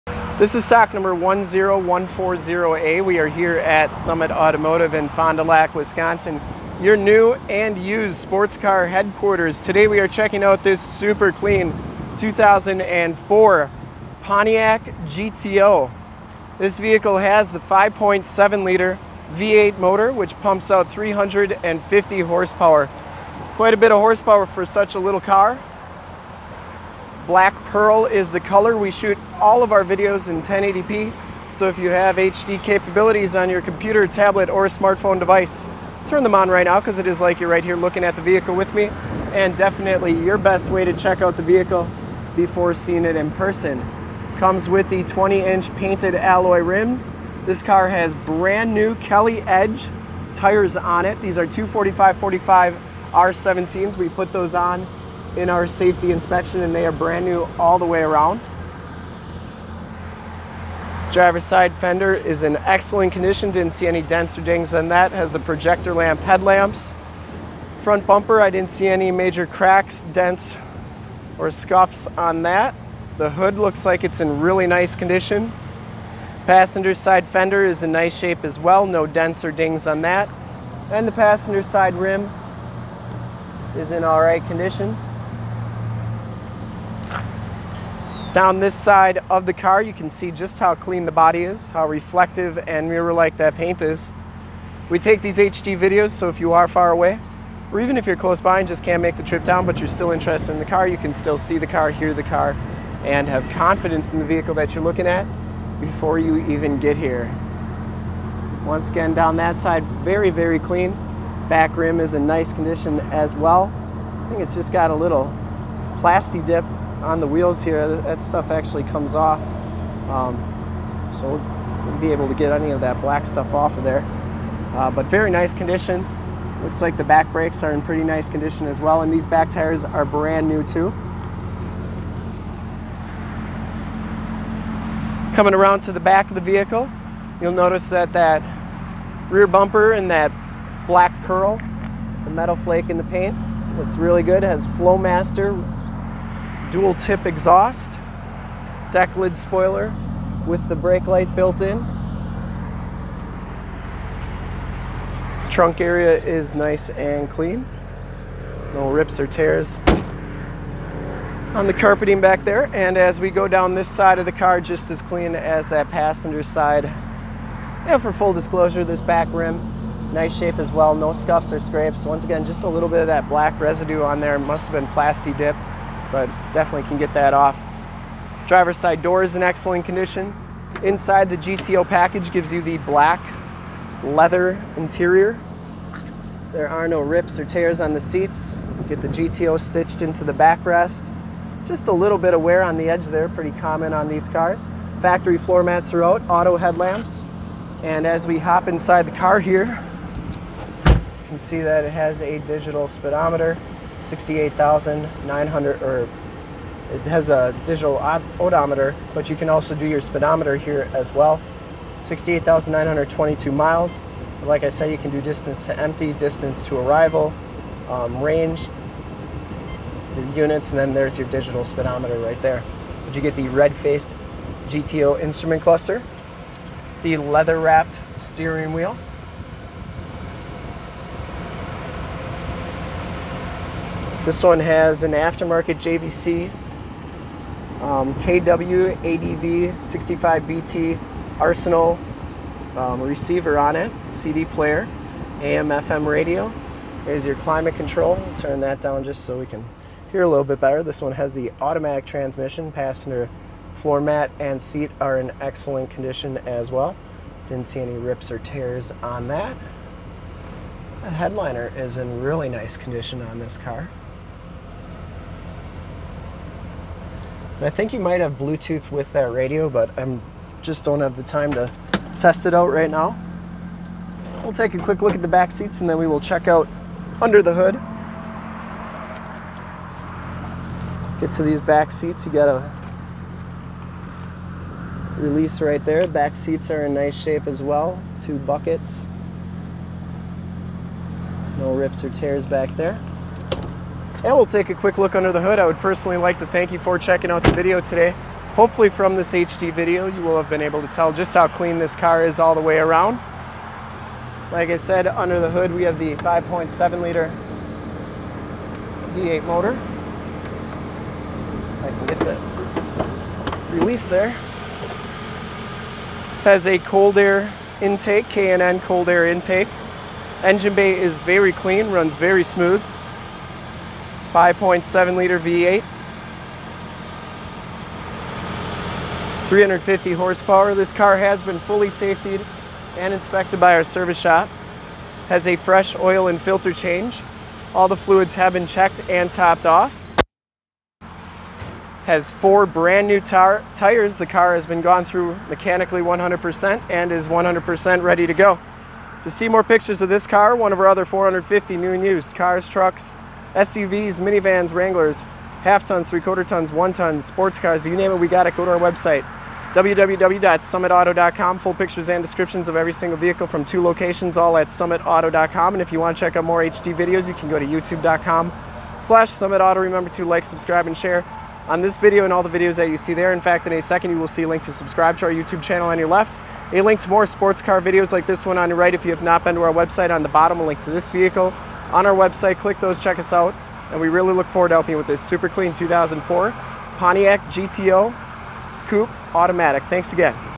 2004 PONTIAC GTO COUPE FOND DU LAC OSHKOSH WISCONSIN 54935 SOLD! 10140A
2004 PONTIAC GTO COUPE IN BLACK

2004 PONTIAC GTO COUPE IN BLACK

STOCK: 10140A
PRICE: $13,999
MILES: 68,893
MAKE: PONTIAC
MODEL: GTO
VIN: 6G2VX12G94L183007
LOCATION: FOND DU LAC OSHKOSH WISCONSIN, 54937 TRUCKS ON 41

CLEAN TITLE HISTORY! 5.7 Liter V8 Engine, 350 Horsepower, Two Door Coupe, Base Two Door Coupe Package, Automatic Transmission Center Console Shifter, Rear Wheel Drive RWD 4x2 Two Wheel Drive, Dual Power Seats, Black Ebony Leather Seats, Bucket Seats, Power Mirrors, Flowmaster Exhaust, Brand New Kelly Edge 245/45 R17 Tires, Painted Alloy Rims Premium Wheels, Four Wheel Disc Brakes, Decklid Spoiler with Integrated Light, Fog Lights, Projector Lamp Headlights, Chrome Tipped Exhaust, AM / FM Radio Tuner, JVC KW-ADV65VT Arsenal CD Player, Keyless Entry System, Rear Window Defroster, Adjustable Height Seatbelts, Driver and Passenger Front Air Bags, L.A.T.C.H. Child Safety System, Factory Floormats, Air Conditioning AC, Cruise Control, Power Locks, Power Windows, Automatic Headlights Autolamp, Tilt/Telescope Steering Wheel, Phantom Black Metallic, CLEAN AUTOCHECK! This is a whole lot of car for not a lot of money! Make your move before this super clean ride is gone!

STOCK: 10140A
PRICE: $13,999
MILES: 68,893
MAKE: PONTIAC
MODEL: GTO
VIN: 6G2VX12G94L183007